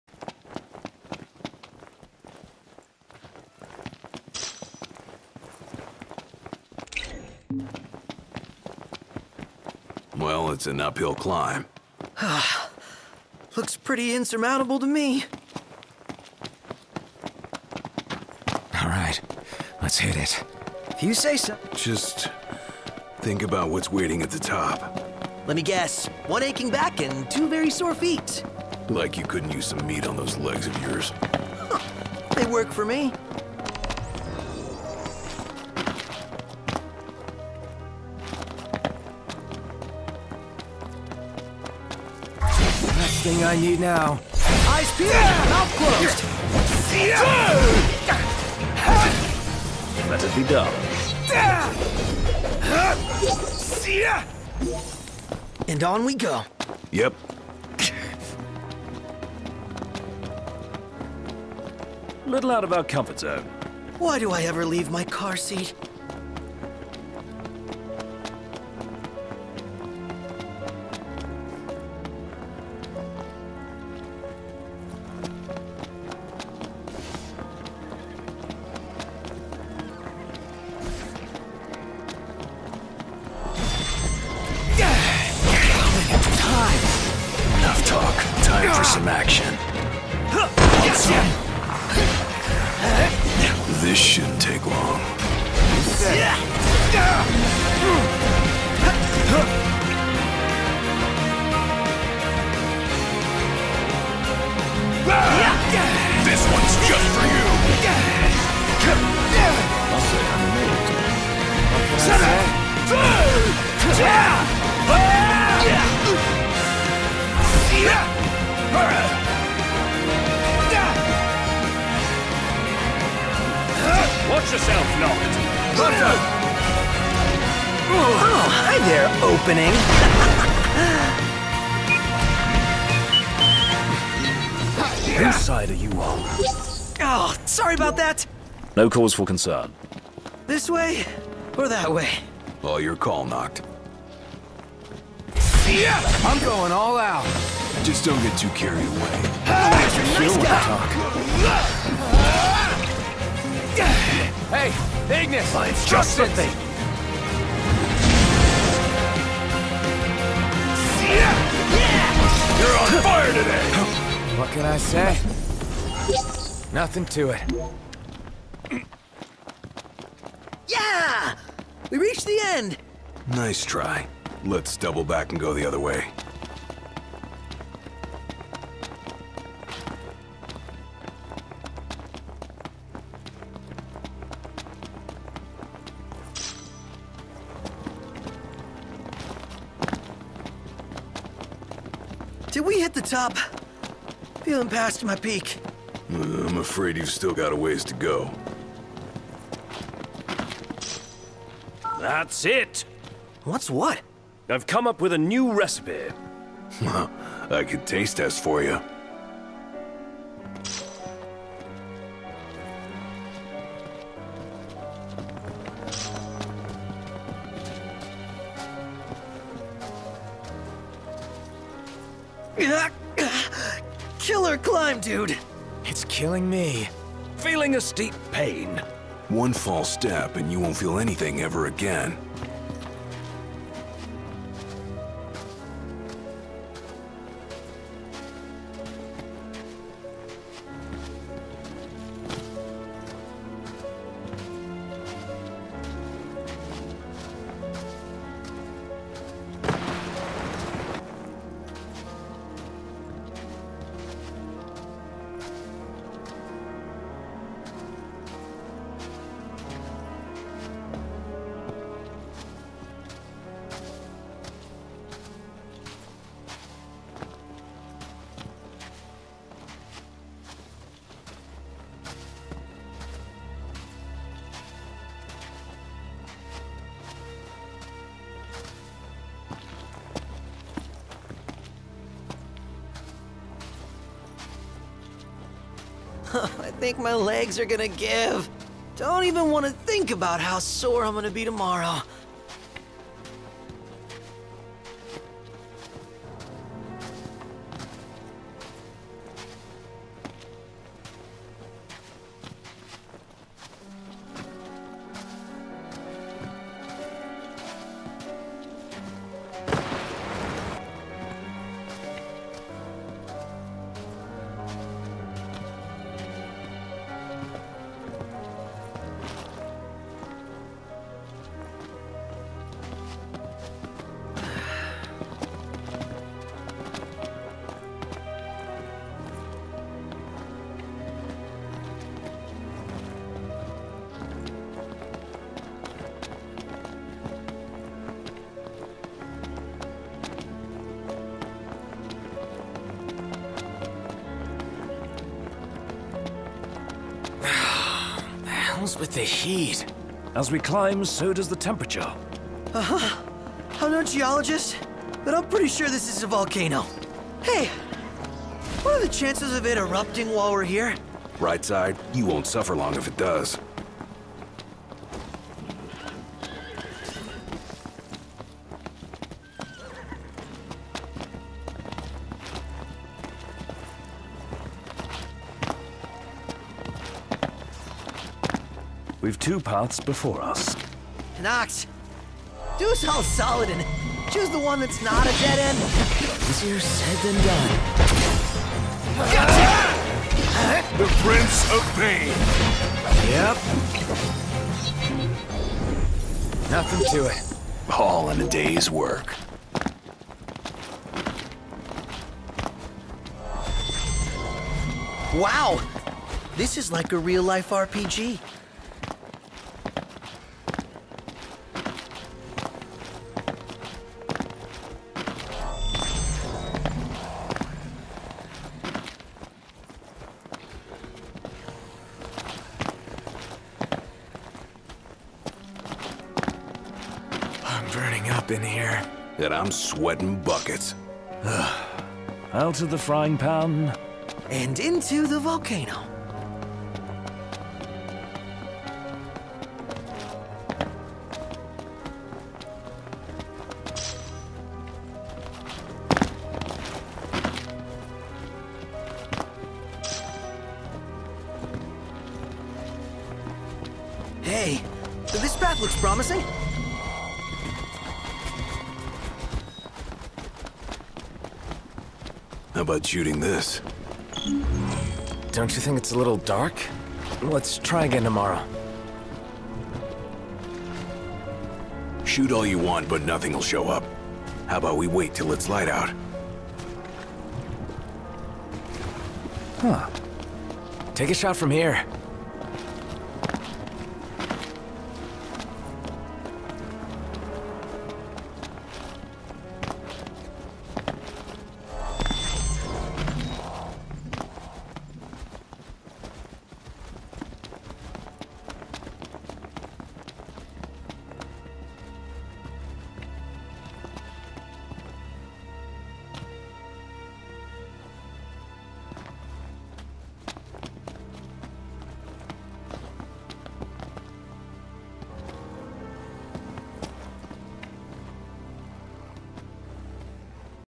Volcano pt. 1
So...I climb this volcano. And then I forget that Vyv's missions can only be completed in broad daylight. :V